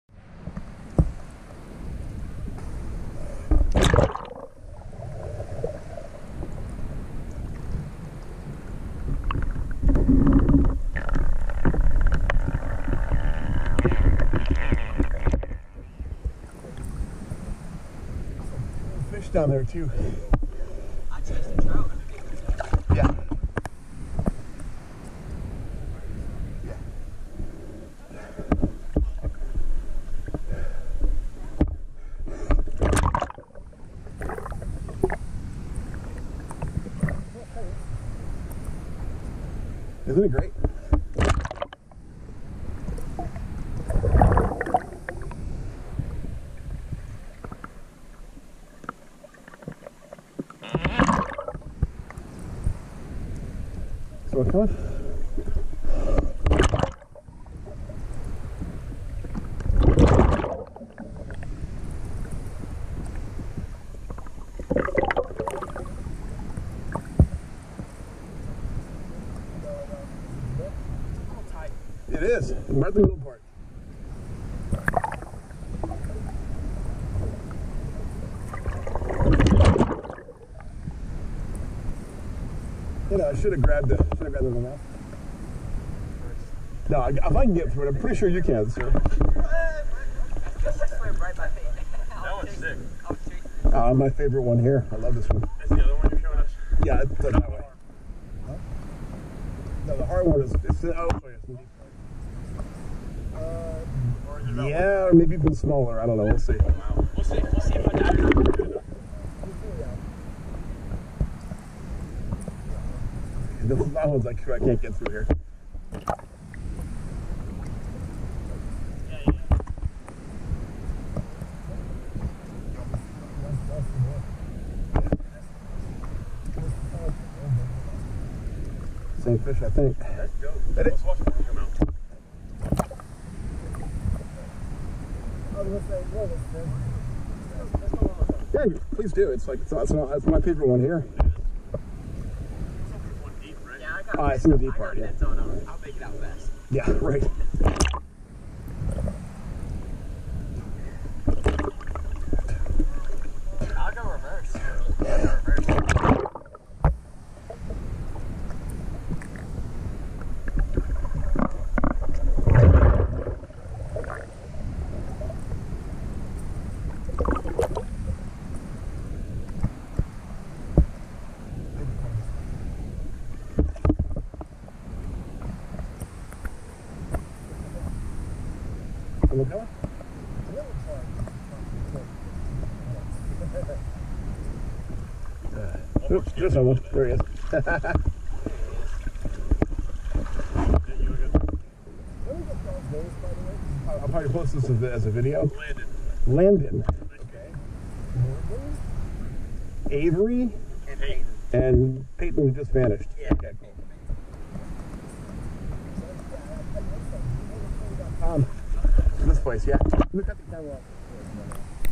Blue Hole Ocoee June 6 2023: at the tilted-rock tunnel with Landing, Morgan, Avery and Payton
What a difference two weeks and about 12 degrees Fahrenheit makes!   With the water 78F upon arrival and 82F when I left six hours later, this was a much warmer visit to Blue Hole and there were actually other people swimming this time!
Here, I show four cool guys  -- Landing (pronounced :Landon"), Morgan, Avery and Payton -- the tunnel under the tilted rock, which is fairly long and a little challenging.  But if I can make it through, almost anyone can, since I'm by no means small.  :)